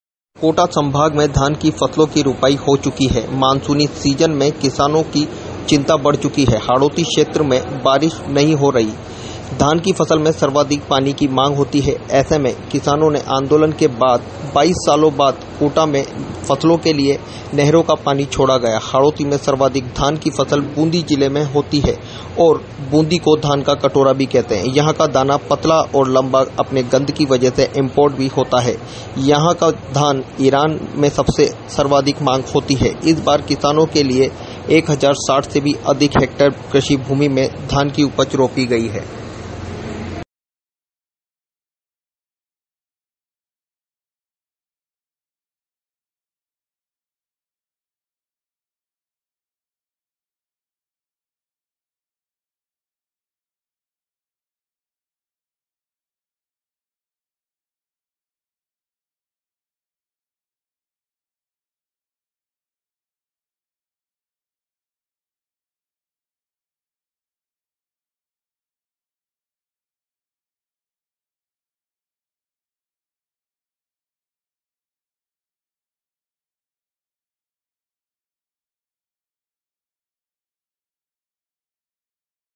राजस्थान के हाड़ौती में धान की मांग में वृद्धि, मानसून सीजन में किसानों को चिंता..
Kota: Rajasthan के हाड़ौती क्षेत्र में धान की फसल की रुपाई हो चुकी है, लेकिन मानसूनी सीजन में किसानों को चिंता सताई हुई है. धान की फसल में सर्वाधिक पानी की जरूरत होती है. ऐसे में कोटा, बूंदी, बारा, झालावाड़ जिलों में मानसून सीजन में अभी भी बारिश नहीं हुई है. राजस्थान के हाड़ौती जिले में सर्वाधिक धान की पैदावार होती है. हाड़ौती का धान पतला और अपनी सुगंध की वजह से बाहर इंपोर्ट भी होता है.दरअसल ईरान में सर्वाधिक मांग बूंदी के धान की होती है.